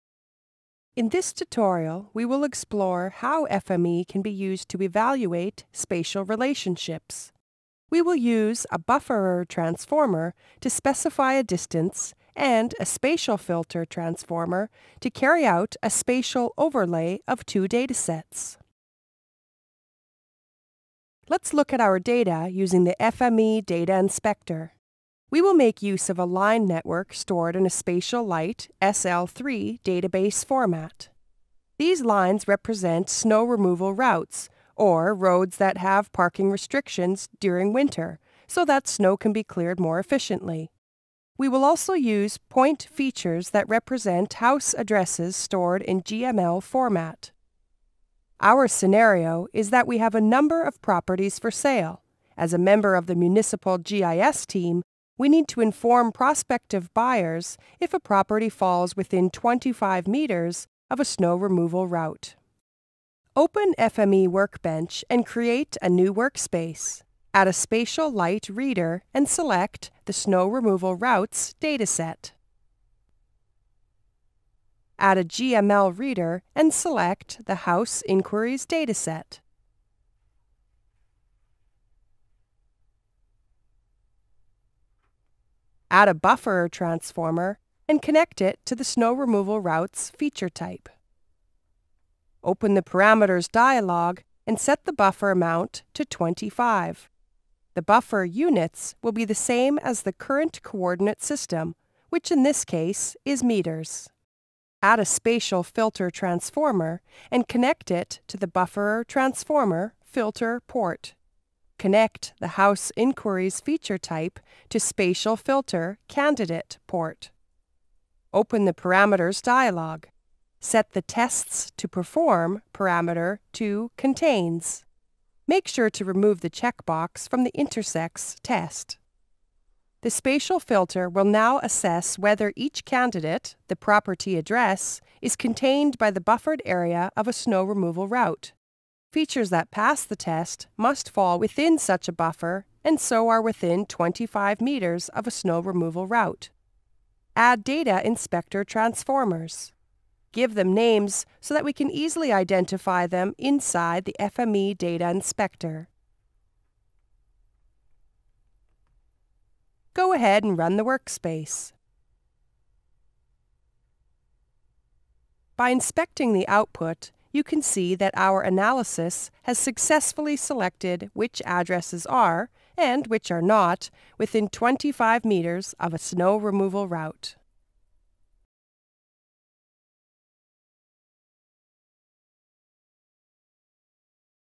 How to test the proximity of spatial features using the FME Bufferer Transformer
Here's how to test the proximity of GIS features such as points and lines. In this demo we'll use a set of linear road features representing snow clearance routes, and a set of point features representing addresses. To discover which addresses are within 25 metres of snow clearance route, the FME Bufferer and SpatialFilter transformers are used.